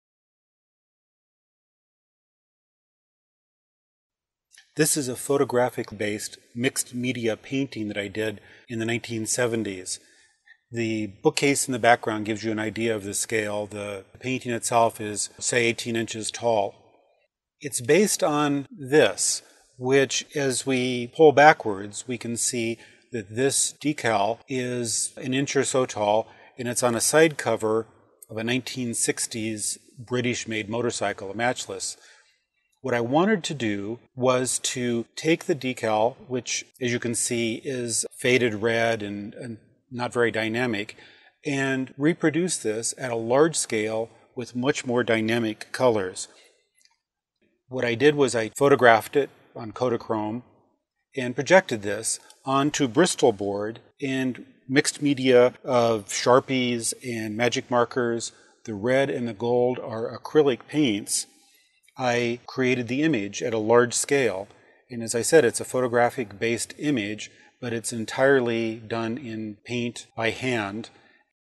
Image Transfer with Projection
In this video, Charles Falco describes how he used projection to enlarge and recreate an image on another surface.